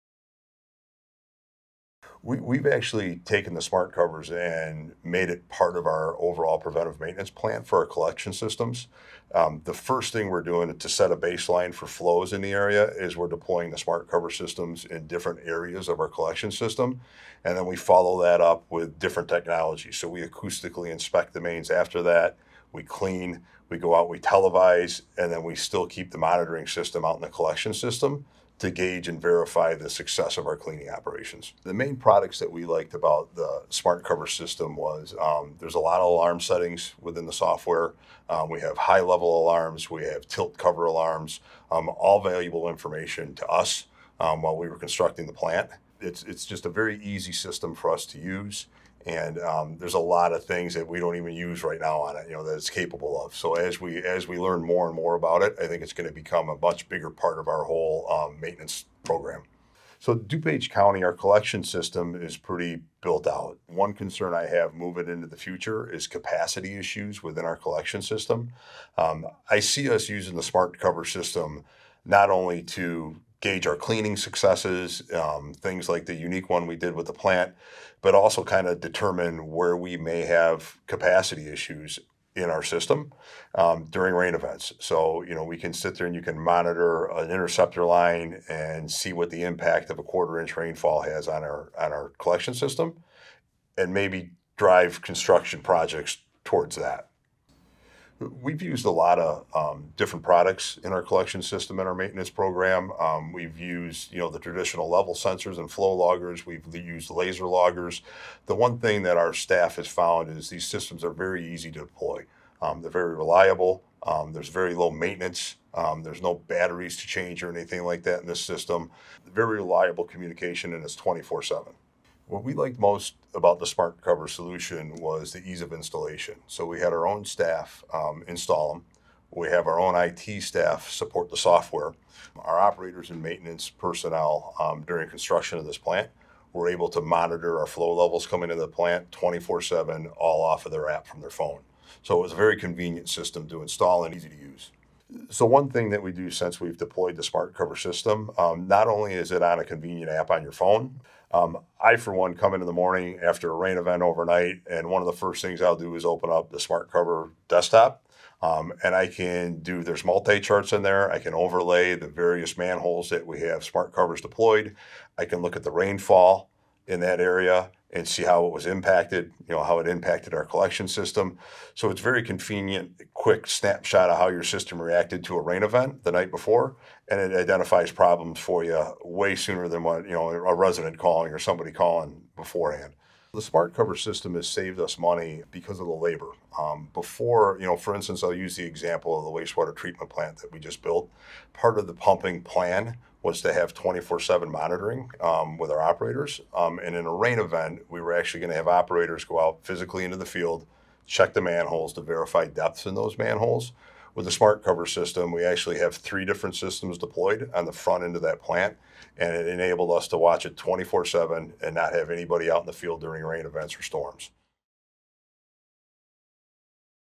DuPage County Public Works Interview
DuPage County Public Works has incorporated SmartCover into their preventative maintenance plan, using its alarm features to enhance cleaning processes and identify potential capacity issues during rain events. The system’s low-maintenance, easy-to-deploy design has improved their collection system management.